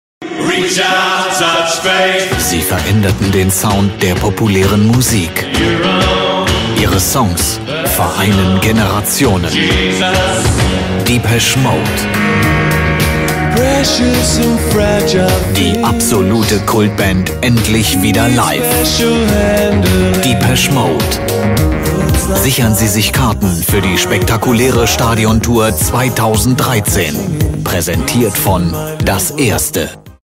Video publicitario: Depeche Mode Tour 2013 (Alemania)
Primer video publicitario del tour 2013. Se confirman antes que la conferencia de prensa se lleve a cabo las fechas en Alemania, con siete conciertos en estadios, en junio / julio.

Munich (01/06.), Stuttgart (03/06.), Frankfurt (05/06.), Berlin (09/06.), Leipzig (11/06.), Hamburgo (17/06.) Y Düsseldorf (03/07.).

.::Marek Lieberberg::.